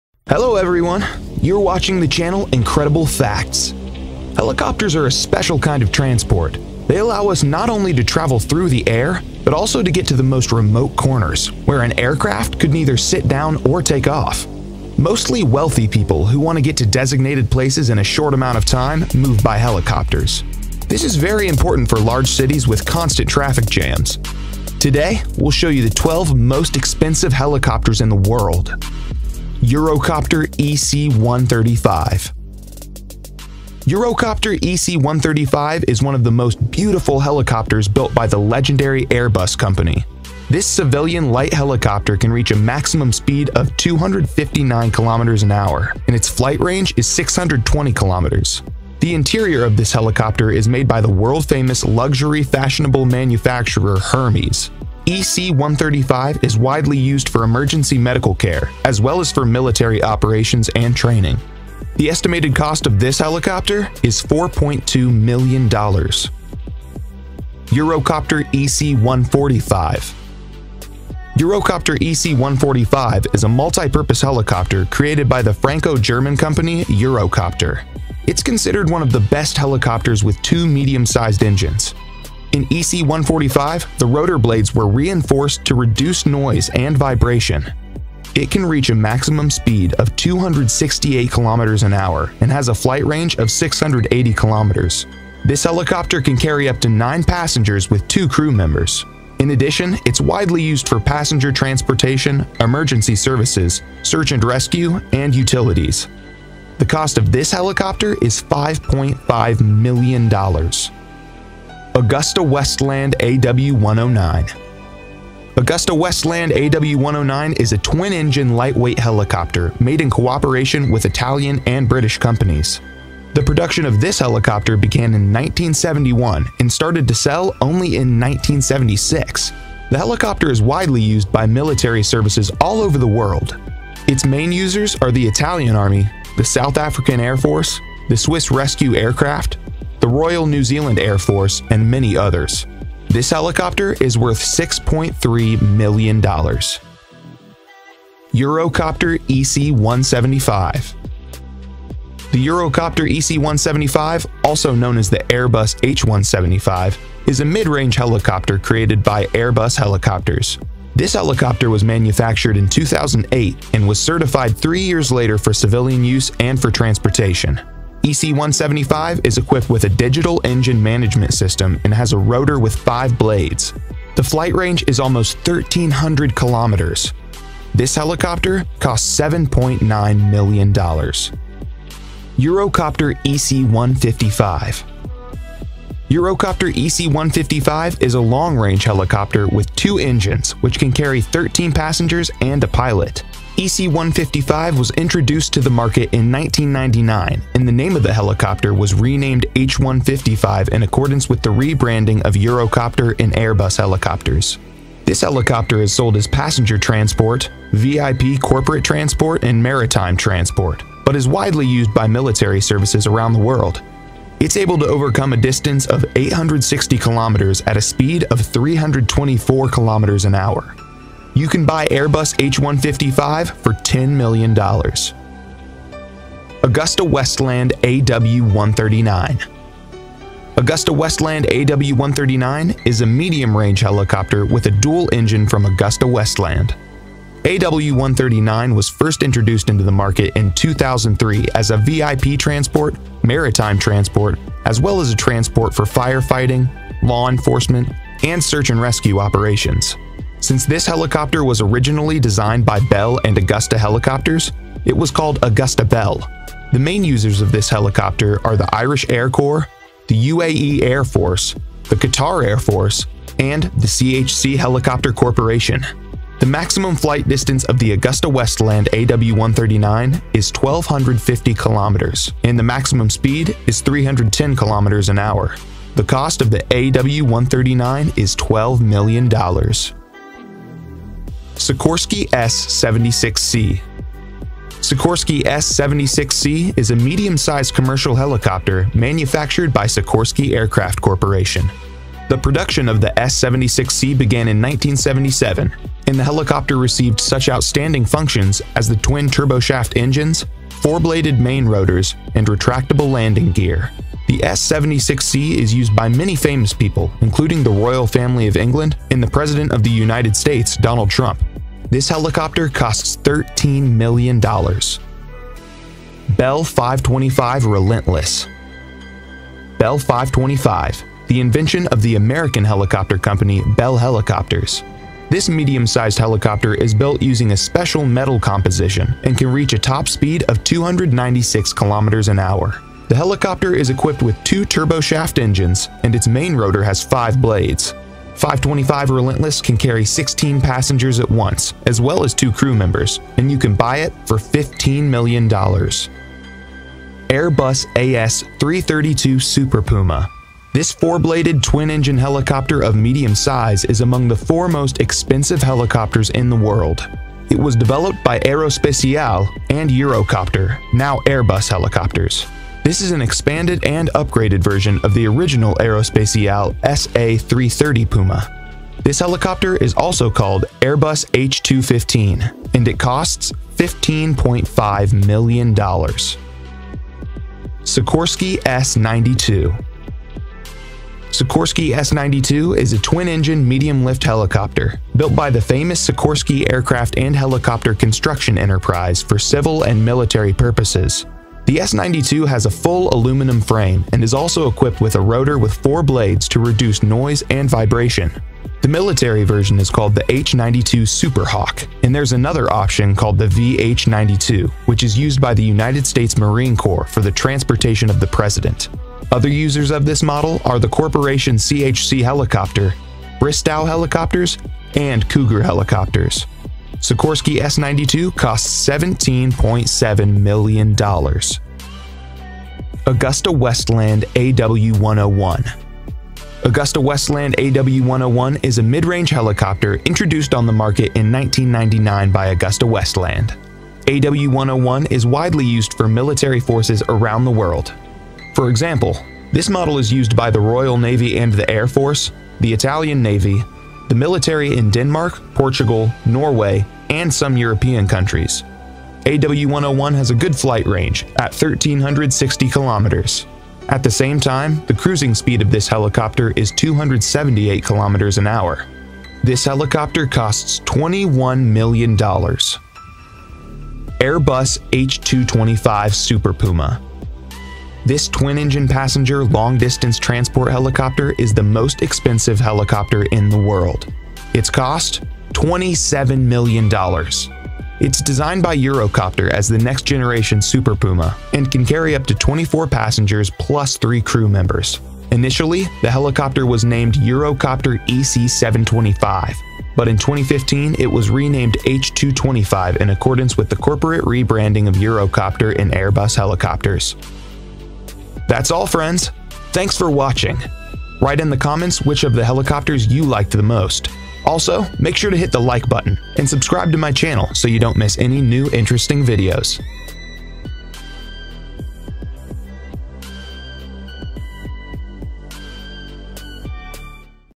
Top 12 best and expensive helicopters that most Africa president can't buy
tell me what  you think if your president buy this 12 best and expensive helicopters.
Do you think President Buhari can buy all this helicopters at once.
and which one do think your president you should buy.